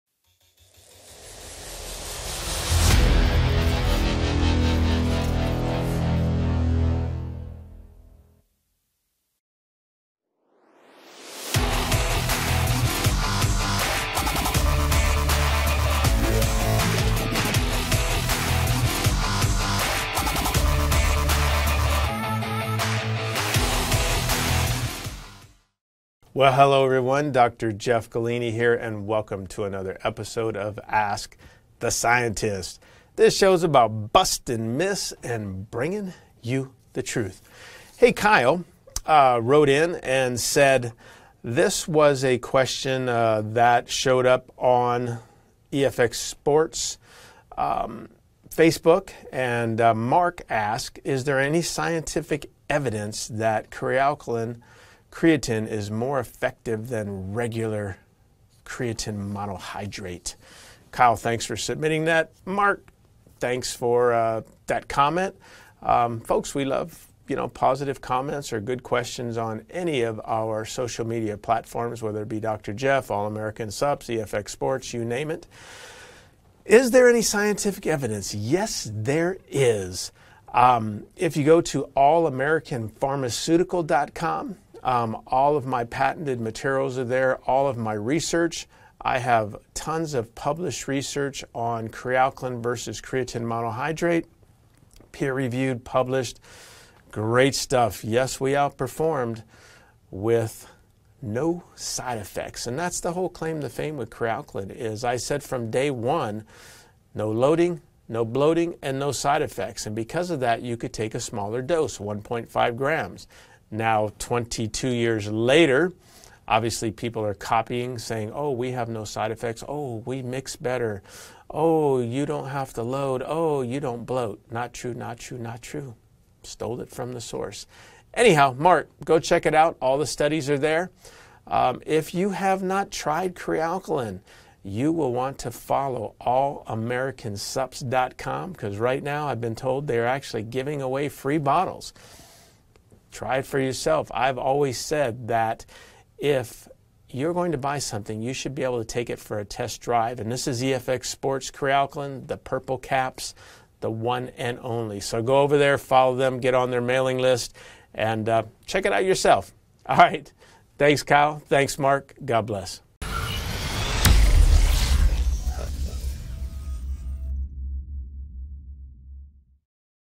Ask The Scientist #56: Is Kre-Alkalyn® Better Than Creatine Monohydrate?
In this episode of Ask The Scientist, Dr. Jeff answers a question that came in about whether or not there's any published evidence that Kre-Alkalyn® is better and more effective than normal creatine monohydrate. Check out his answer!